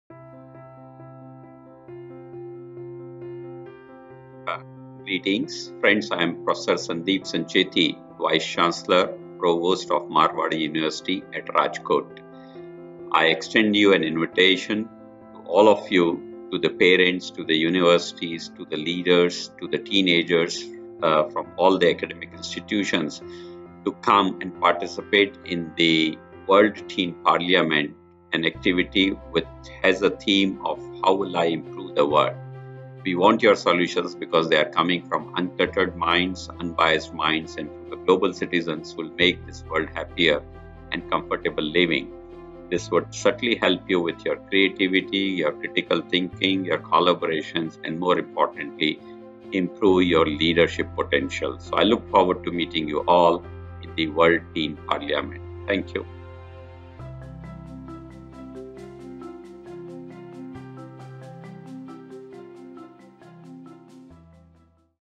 Inviting Teens to the World Teen parliament - Prof. Sandeep Sancheti, VC, Marwadi University, Rajkot
Prof. Sandeep Sancheti, Provost (Vice Chancellor), Marwadi University, Rajkot, Gujarat, India is inviting teenagers from all across the world to participate in the World Teen Parliament, with their ideas and innovations to make the world a better place to live.

World Teen Parliament is the world's first exclusive teen parliament, with a focus on teens driving change for a better future.